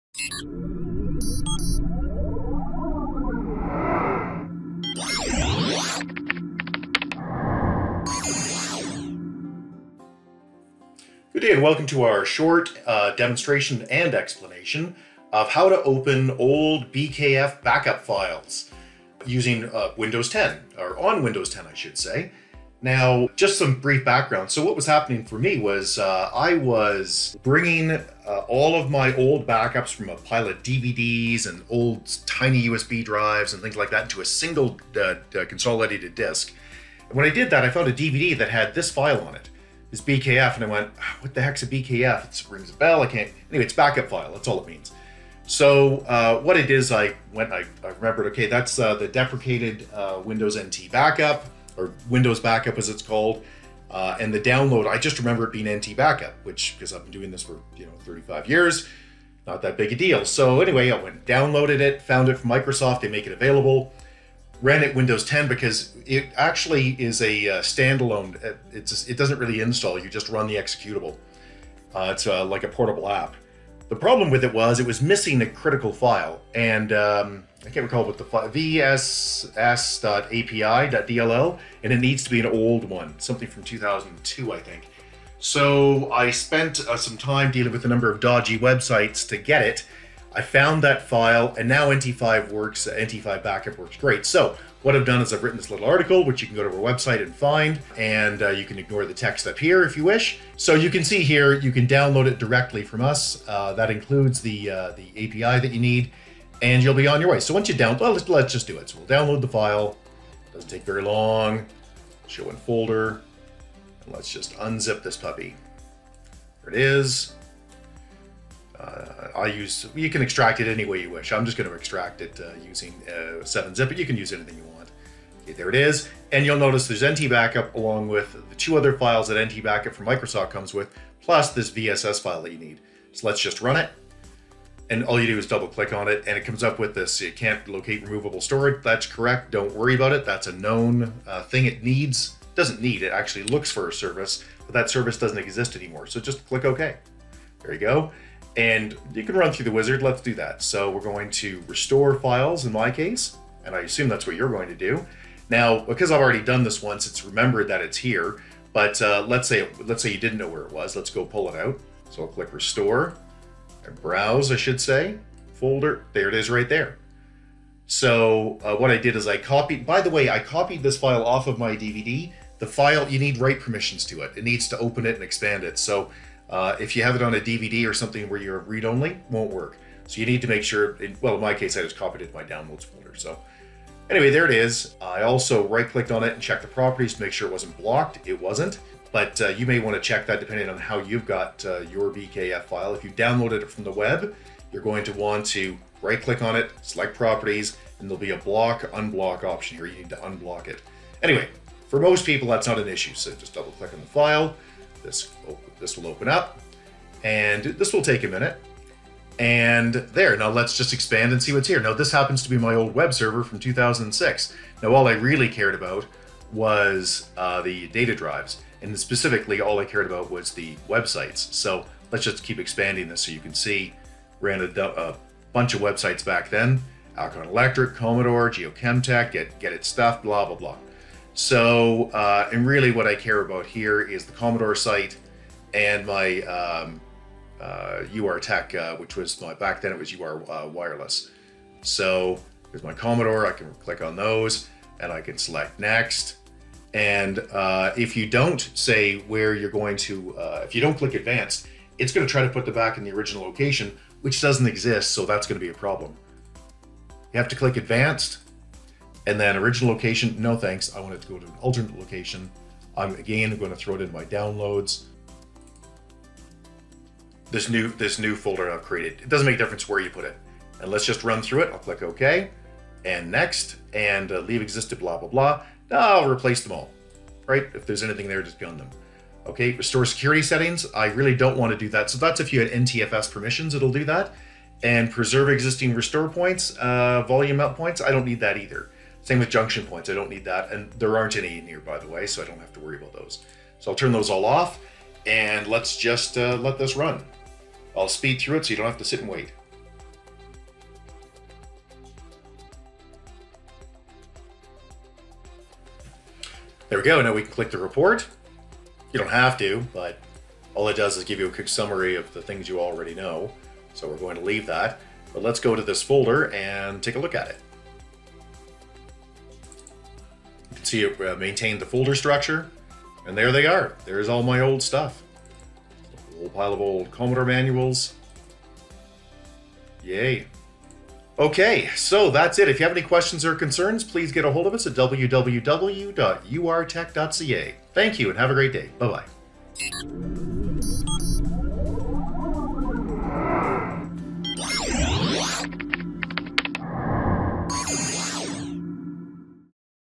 How To Open, Extract & Restore .BKF Windows Backup Files in Windows 10
We explain and demonstrate that you can download a working free version of Windows Backup from our and use it to open and restore files from .BKF files.

.BKF's are backups made with Windows Backup (aka NT5 Backup) which was deprecated and removed from versions of Windows after Windows XP.